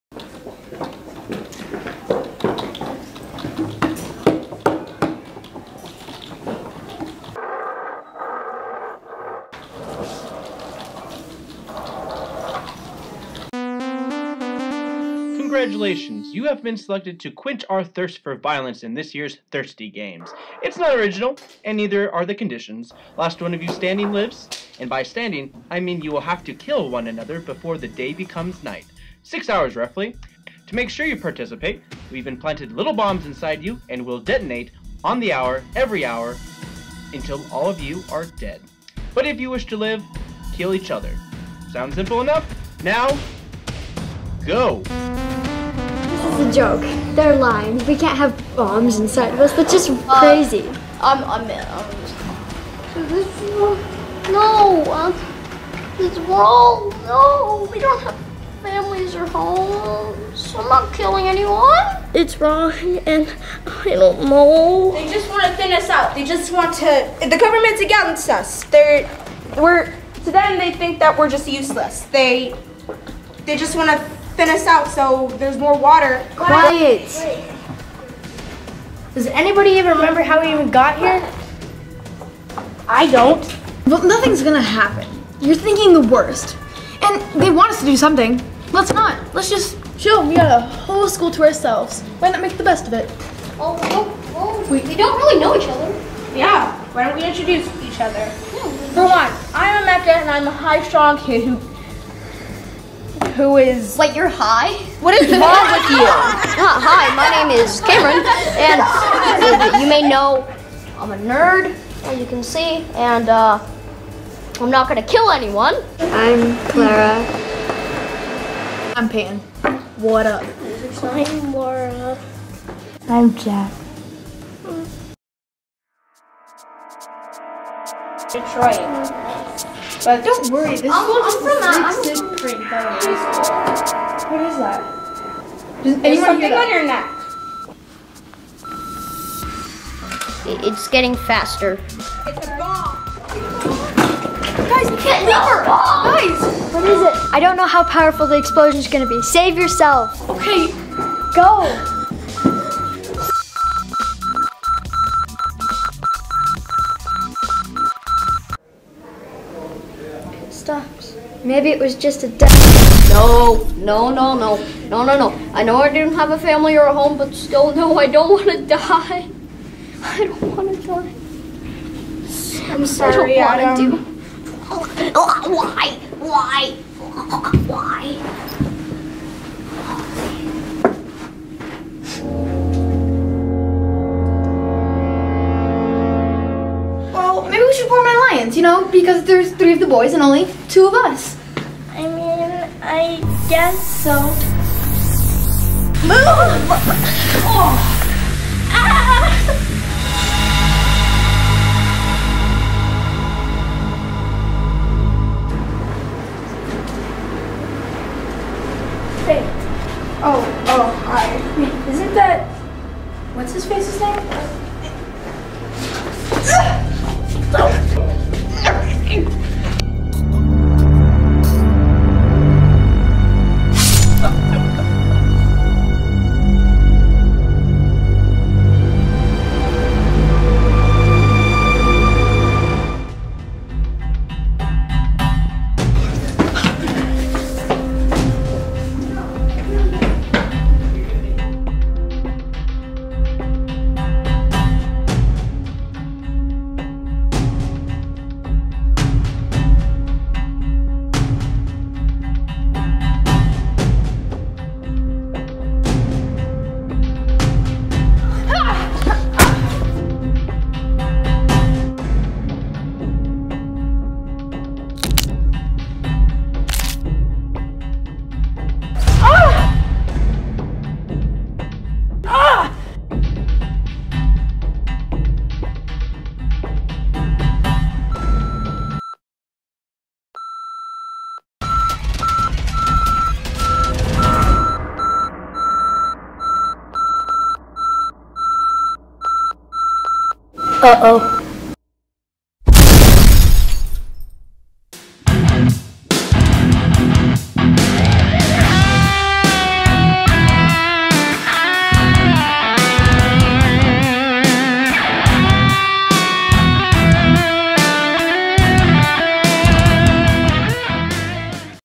Flagship Friday: Thirsty Games
The kids from our local are schools make a battle royale type movie. The Sequel reunites the same cast from some reason.